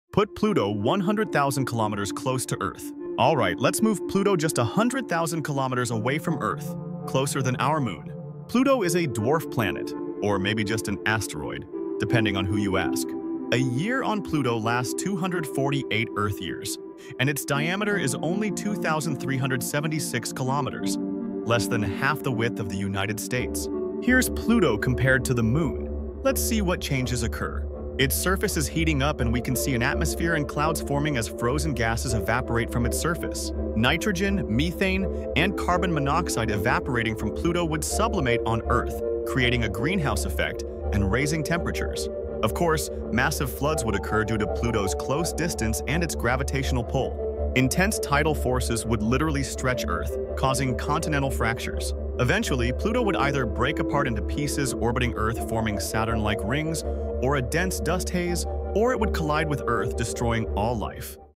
What If Pluto 100,000 km from Earth?☄️🌍spacesimulation
What would happen if Pluto suddenly appeared just 100,000 km from Earth?
In this simulation, we explore the unimaginable consequences of having the dwarf planet so close to our planet.

Pluto’s surface heats up, releasing nitrogen, methane, and carbon monoxide into space.

These gases sublimate on Earth, creating a massive greenhouse effect and rising temperatures.

Intense tidal forces stretch the Earth, causing continental fractures.

Floods, atmospheric chaos, and eventually either a collision or Pluto breaking into Saturn-like rings.

Experience a thrilling journey through space and see how a seemingly small change could completely reshape our planet!

🌌 Made with Universe Sandbox